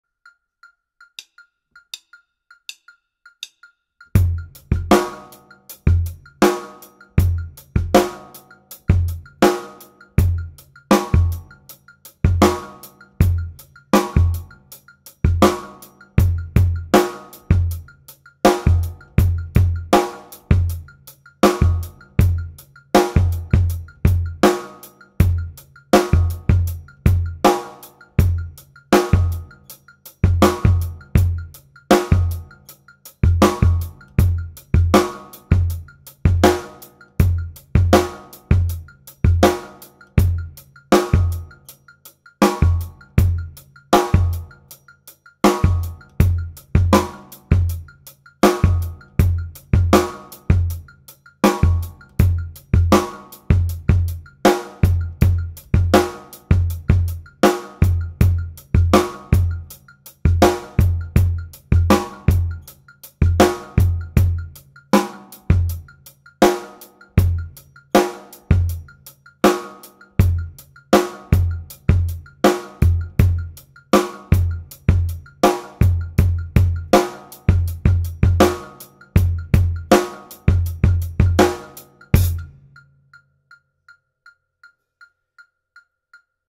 The Breakbeat Bible e2 page18 80bpm
Jouez avec moi la page 18 à 80 bpm, deux fois chaque mesure.

Métronome sur les double-croches "en l'air".